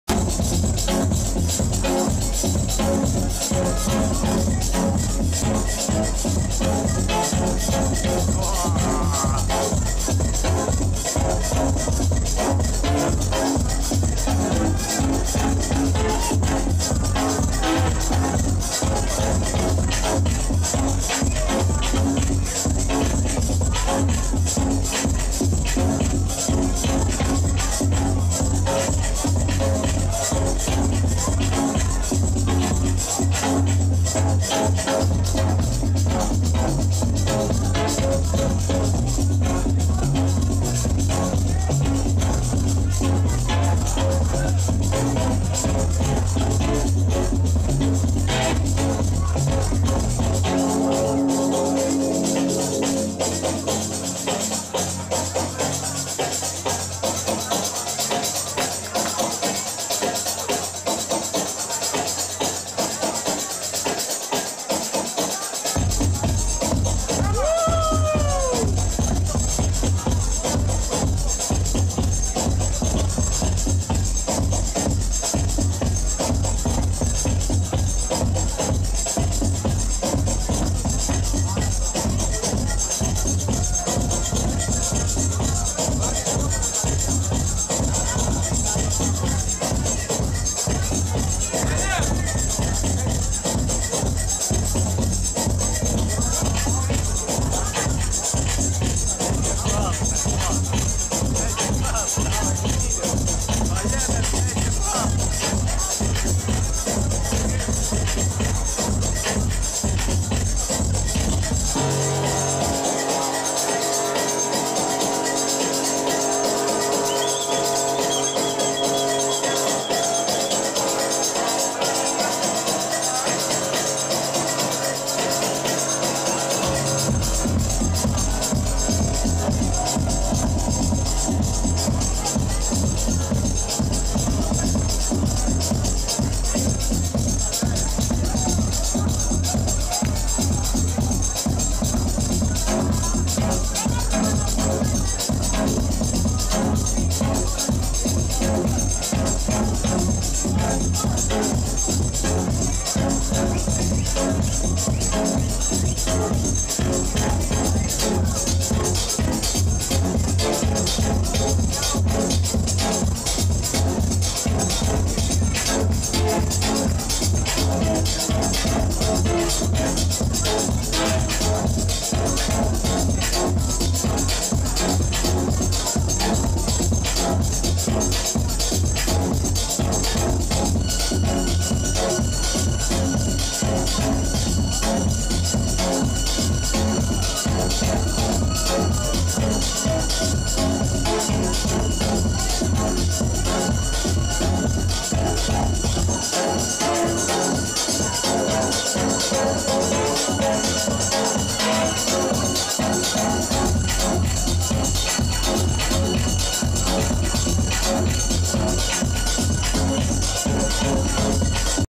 BIG FISH with Fabrizio Maurizi @ Club AQUARIUM London 05.07.2014
MOVIMENTO  Afterhours
Starting on Saturday night and going trough the late hours on Sunday morning 11 pm  till 9am
Its a mad journey, travelling through all shades of house, tech house, techno , deep disco & electronic soul
Swimming pool open